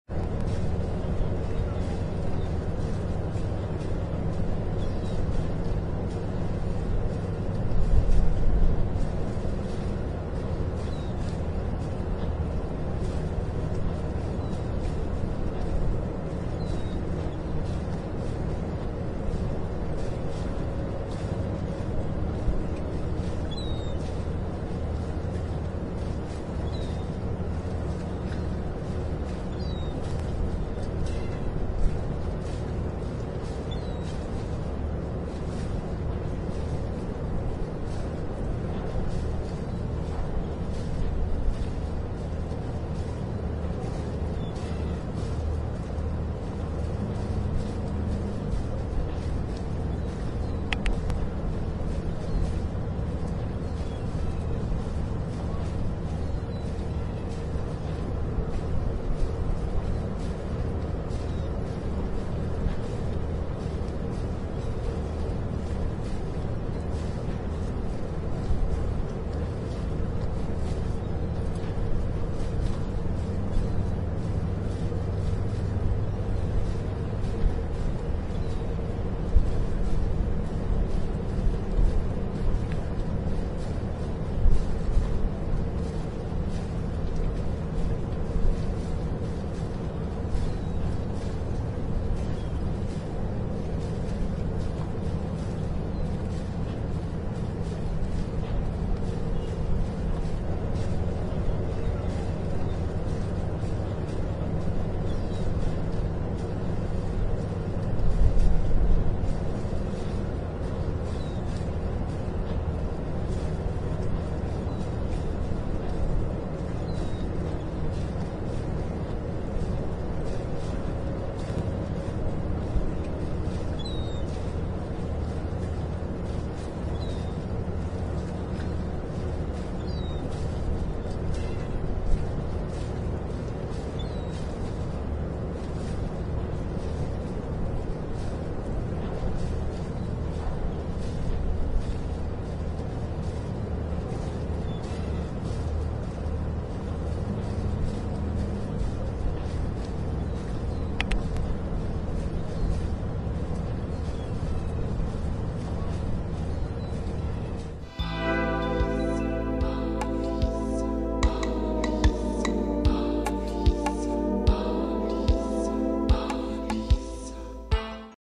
HMM NURI | CUXHAVEN | SHIPSPOTTING | WELTSCHIFFFAHRTSWEG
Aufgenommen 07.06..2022

HMM NURI   Container Ship   Flag: Liberia   16010 TEU
GT: 153150   DWT: 160357 t   366 x 51 m   Year Built: 2021